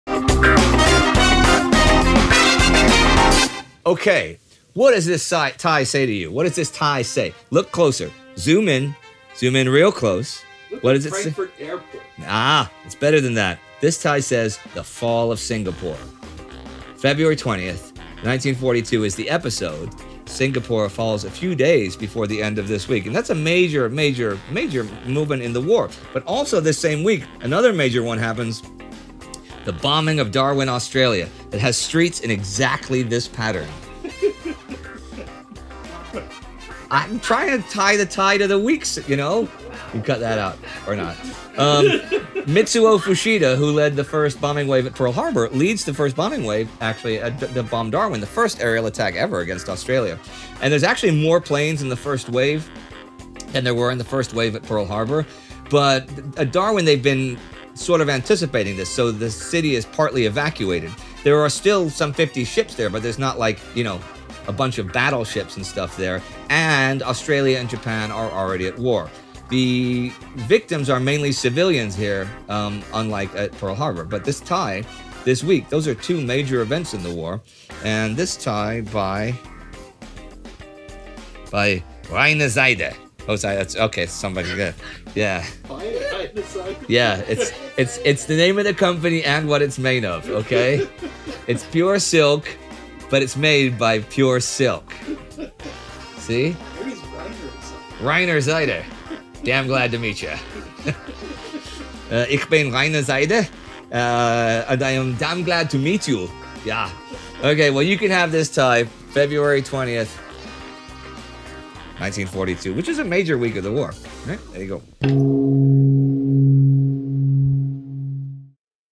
SOLD A Tie to Navigate Darwin and Leave Singapore - WW2 130
A pure silk dream for anyone that appreciates the concept of lines. It's the sort of tie that tells people you know where you've been, and you know where you're going - a bit like Mitsuo Fuchida - he knew sure as hellfire that he had been over Pearl Harbor, and he knew he was going to Darwin. Now he might not have wore this tie when flying to those places, but he probably would have if he could have. So be at straight shooter and acquire this wonderful set of intersecting parallel lines.